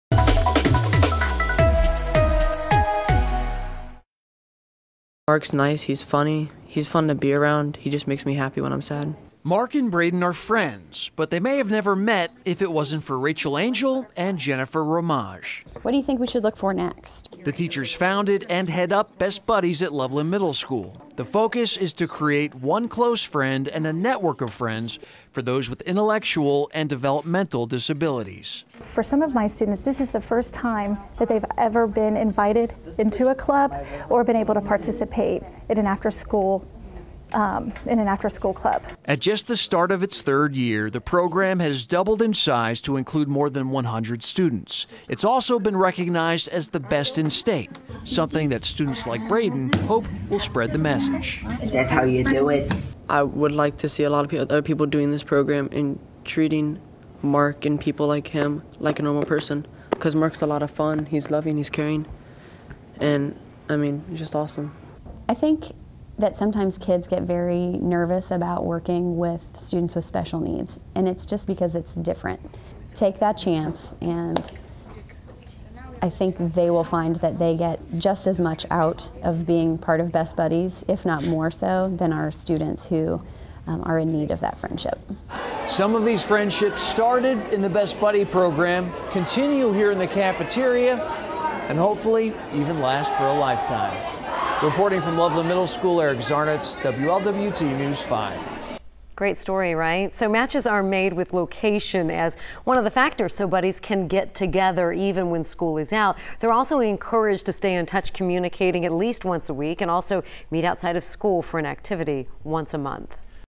Loveland Middle School's Best Buddies program garners state recognition
The Best Buddies program at Loveland Middle School focuses on creating one close friend and a network of friends for those with intellectual and developmental disabilities.